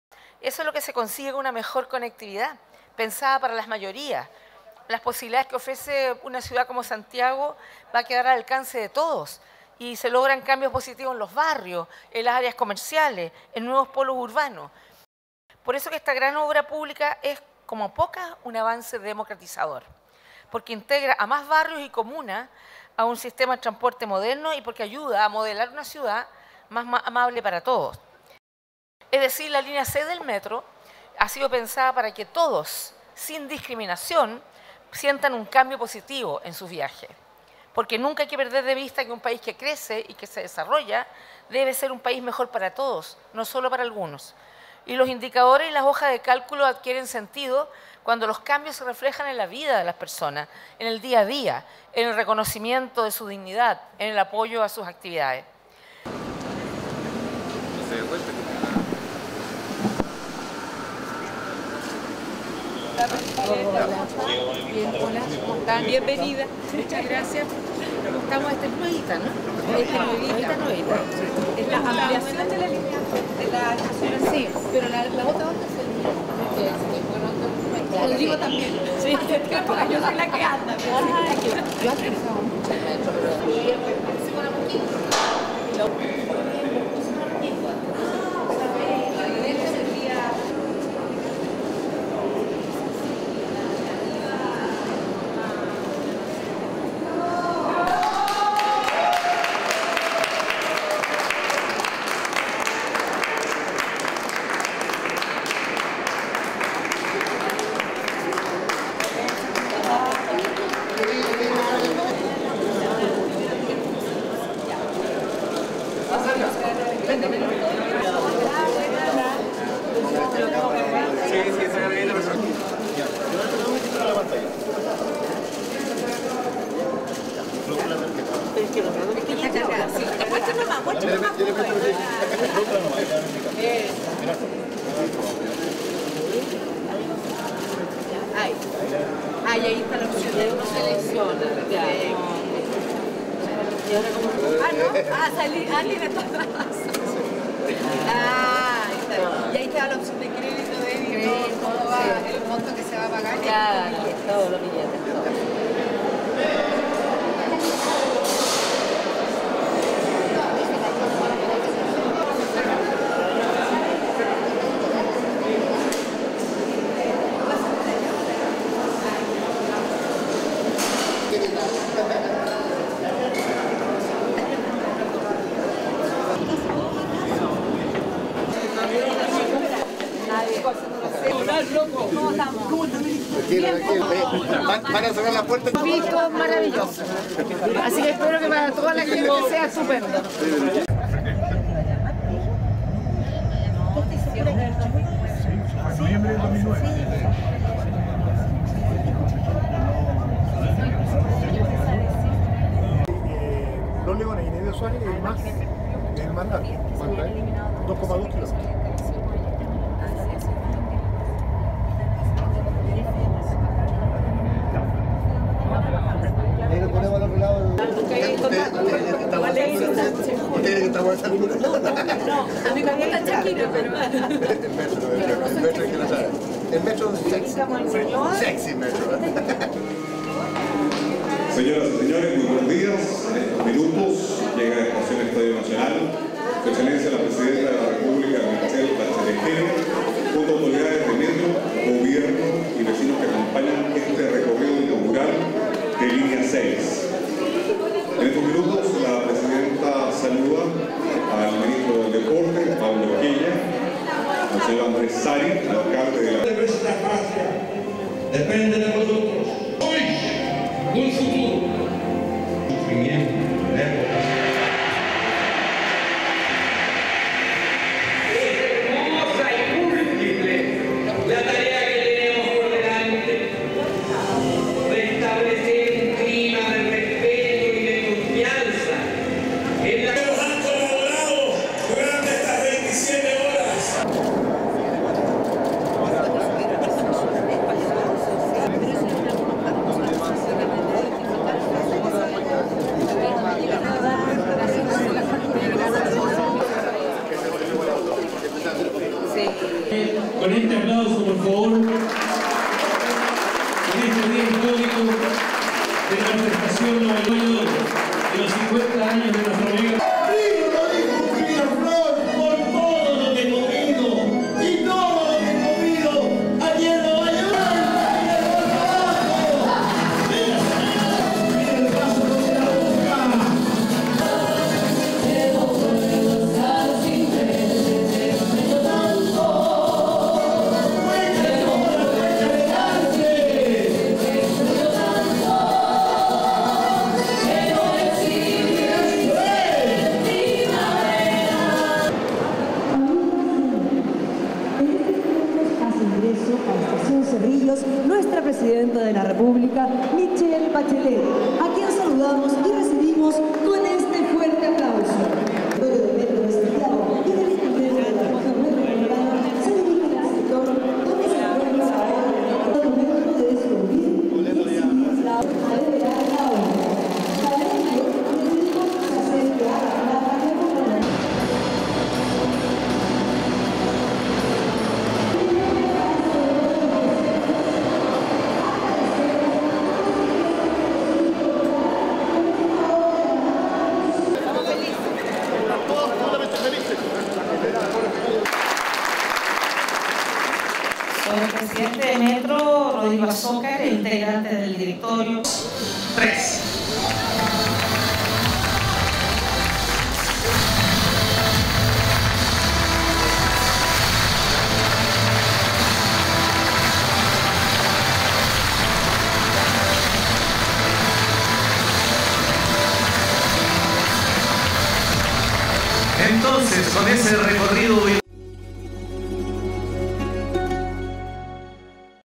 Línea 6 del Metro que beneficiará a más de un millón de personas
Esta mañana, la Presidenta de la República, Michelle Bachelet, inauguró la línea 6 del Metro de Santiago, que representa 15 kilómetros de trazado y 10 estaciones en las comunas de Cerrillos, Pedro Aguirre Cerda, San Miguel, San Joaquín, Ñuñoa, Santiago y Providencia, y que se estima, beneficiará a 1.1 millones de habitantes.
Video : Gobierno de Chile, 2 de noviembre 2017.